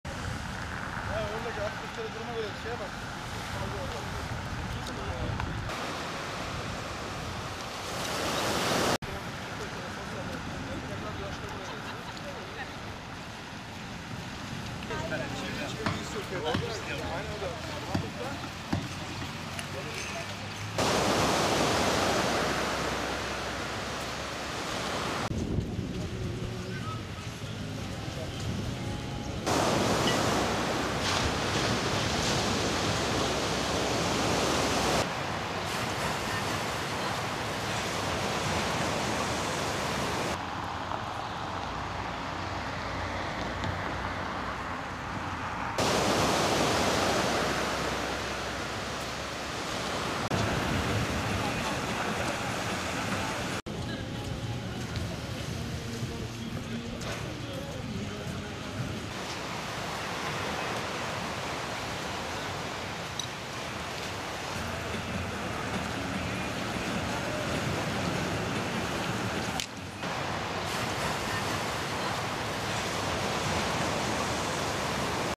ERDEMLİ’DE ÖRNEK DAVRANIŞ
Erdemli Doğa Limon Kulübü (ELİDOK), Erdemli Belediyesi ve Erdemli Kent Konseyi, Erdemli sahilinde çevre temizlik etkinliği yaptılar. Tüm Erdemli sahillerinin temiz kalması konusunda yoğun çalışmaların yapıldığını bildiren Erdemli Belediye Başkanı Mükerrem Tollu, bu konuda sivil toplum kuruluşlarının da destek vermesinin sevindirici olduğunu söyledi.